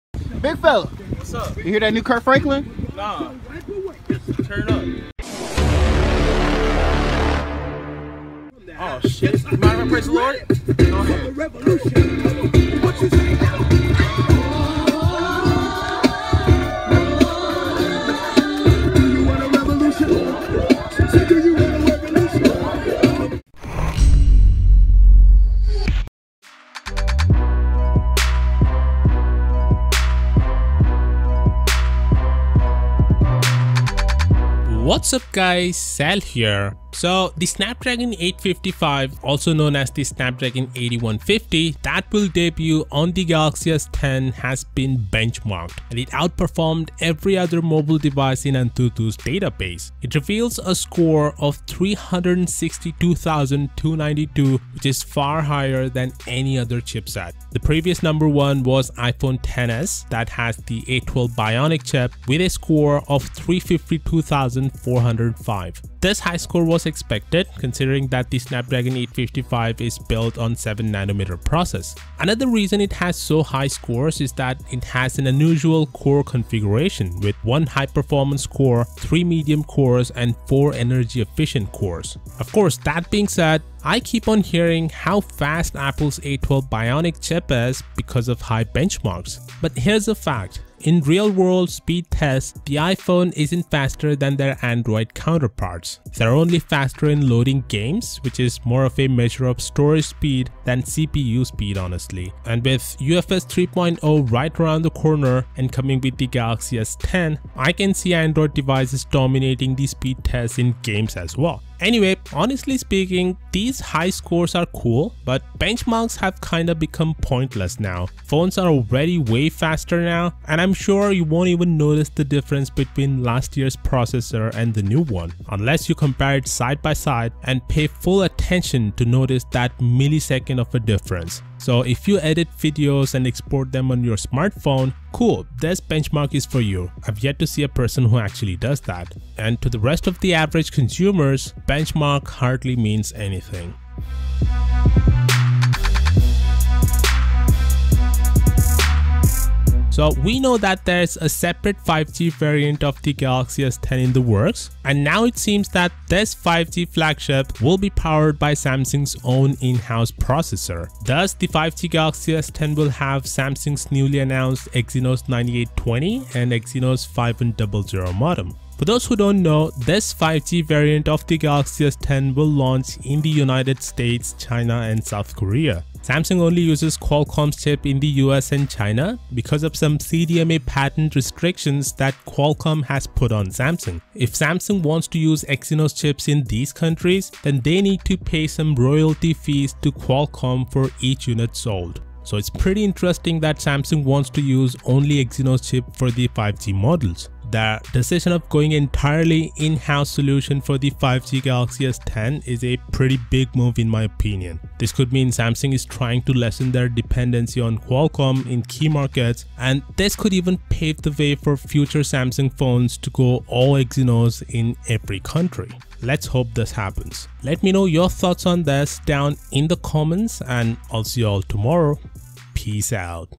Samsung Galaxy S10 - THE CHIP BROKE RECORDS
Samsung Galaxy S10 will come in two variants and the Snapdragon variant will have the fastest processor out there. The benchmark leak shows it broke all previous records. Samsung Galaxy S10 leaks suggest the device will have an ultrasonic FPS. The Samsung Galaxy S10 rumors suggest the device will have a separate 5G variant.